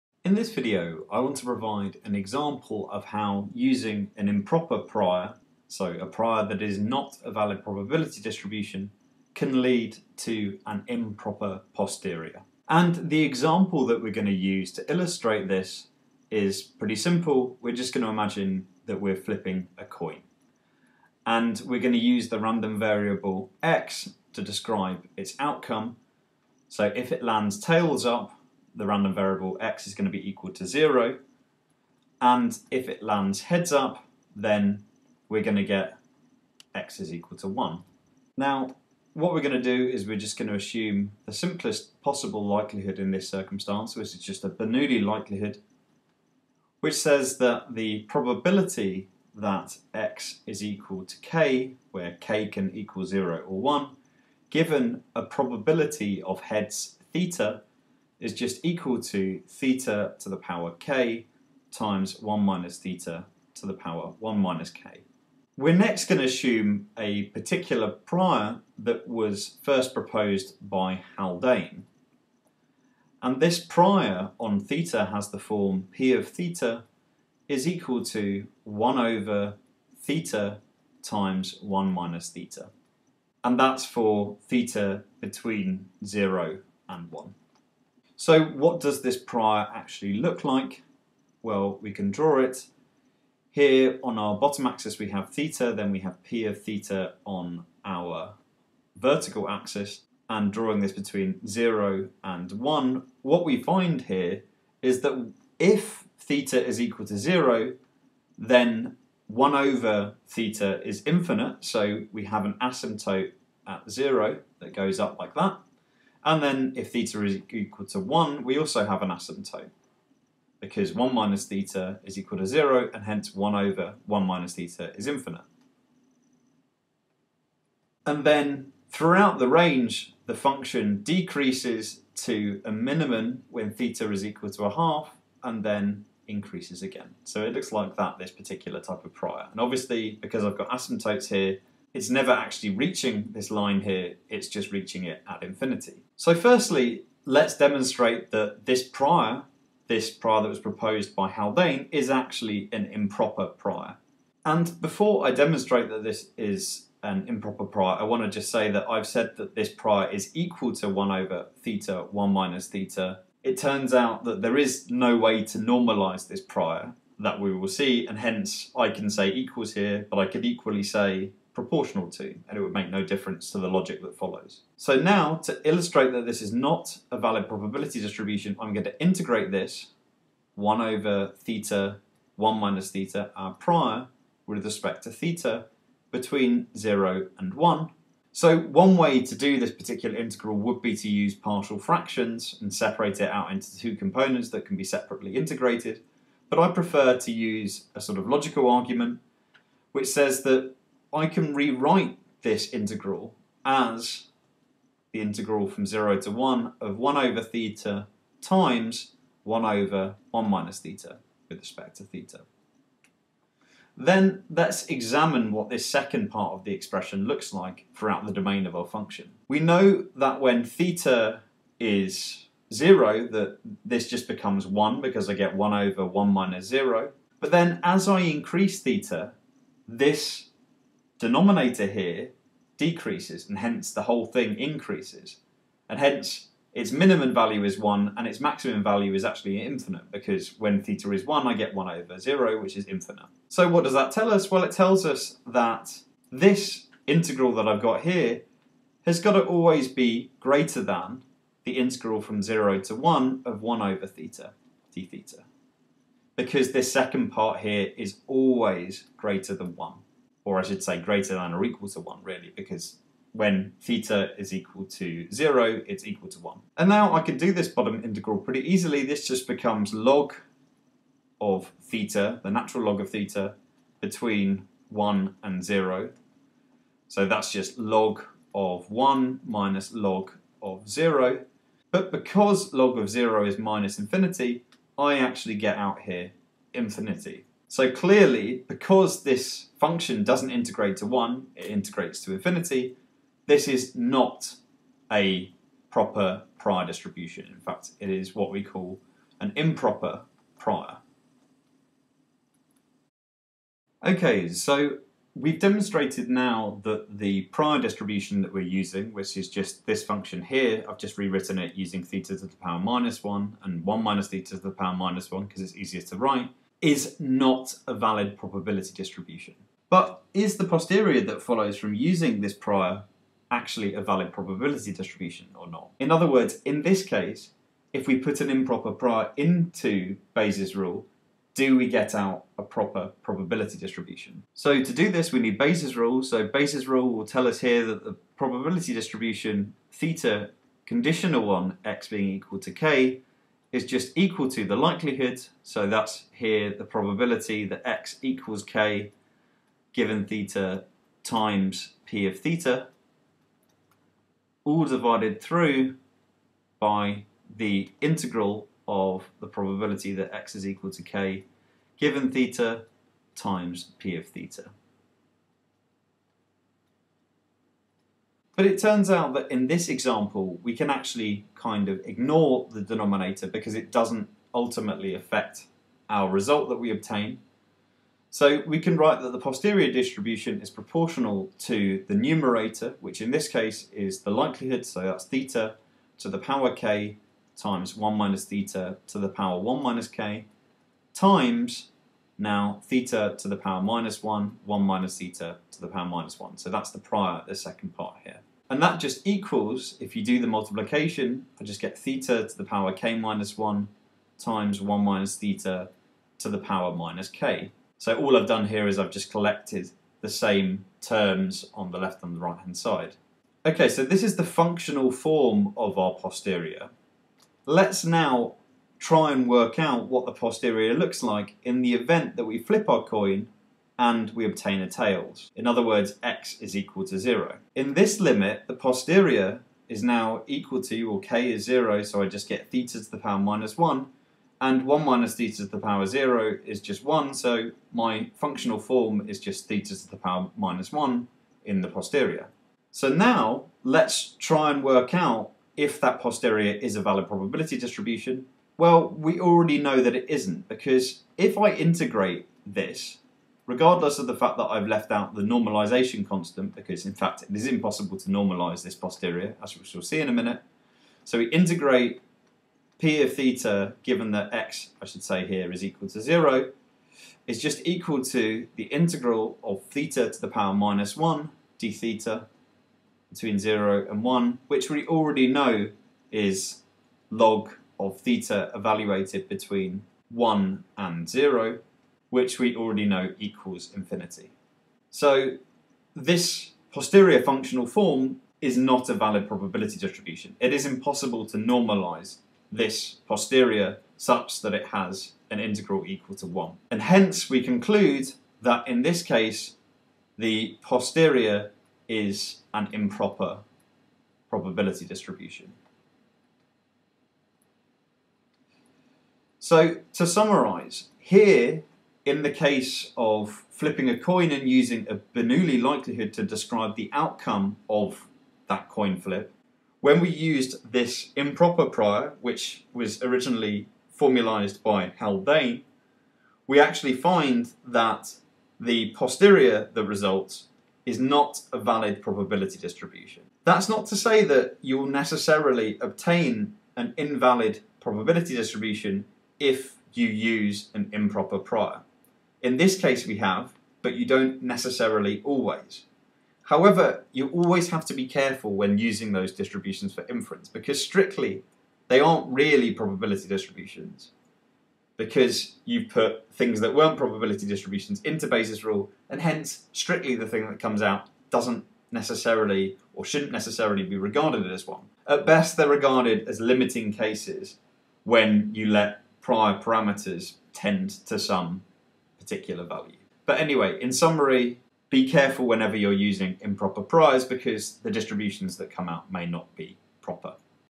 An example of how an improper prior leads to an improper posterior
Provides an illustration of how choosing a prior probability distribution which is not valid can lead to an improper posterior distribution.

This video is part of a lecture course which closely follows the material covered in the book, "A Student's Guide to Bayesian Statistics", published by Sage, which is available to order on Amazon

For more information on all things Bayesian, have a look The playlist for the lecture course is